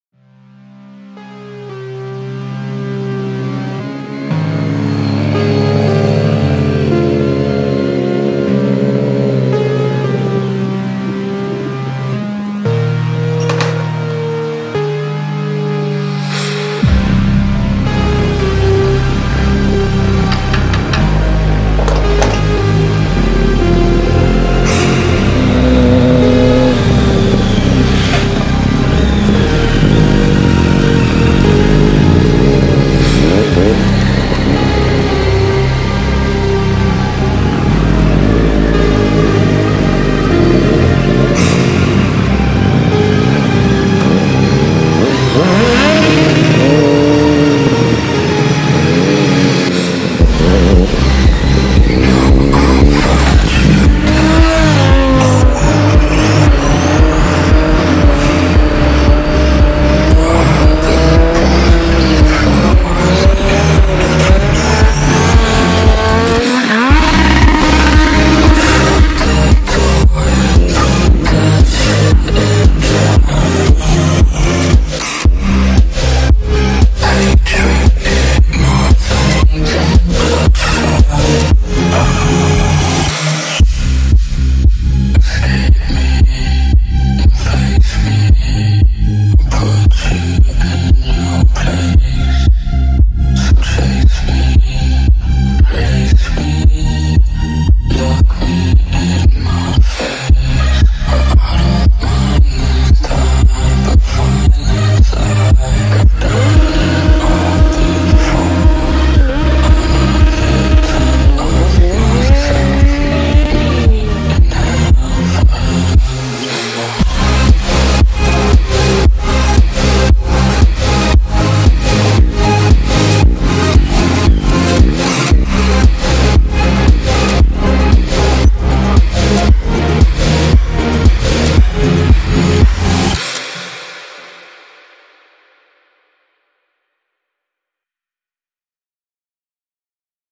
KAROL KULBACKI - TRIUMPH STREET TRIPLE 675
A short film presenting Karol's stunt skills, the film was recorded for a year. Sorry for the incompatibility of shots. Have a nice watching :D

Video: @mkvvid
Rider: @karxr6